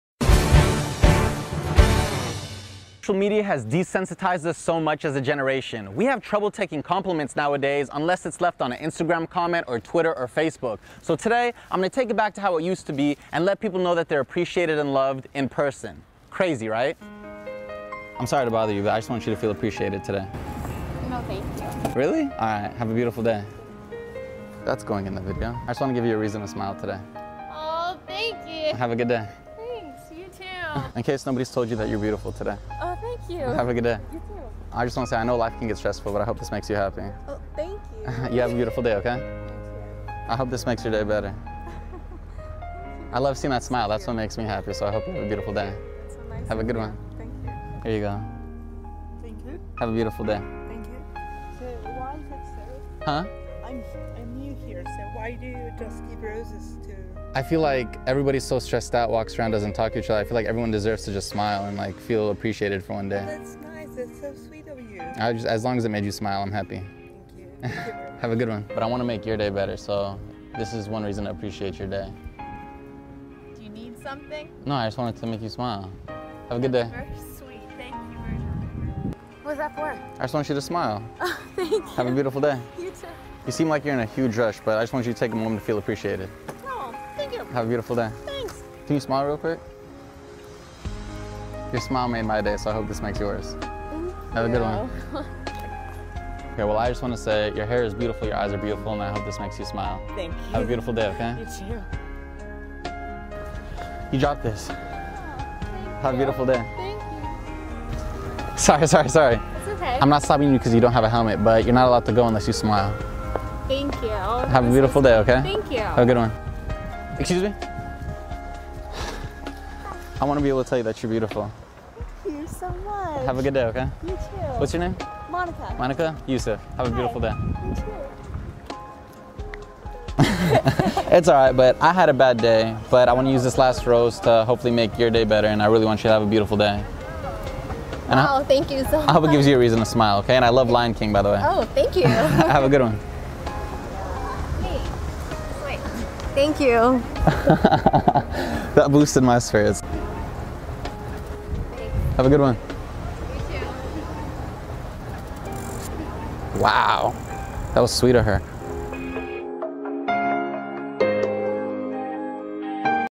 A Reason to Smile, Grande Prairie Auto Group
Wouldn't it be nice if someone from Grande Prairie AB walked up to you and handed you a rose, just to make your day? I think the Peace Country is full of kind spirited people, we can each make a difference in our community.

Copyright to: fouseyTUBE  Published on 27 Apr 2014
Interact with me on these sites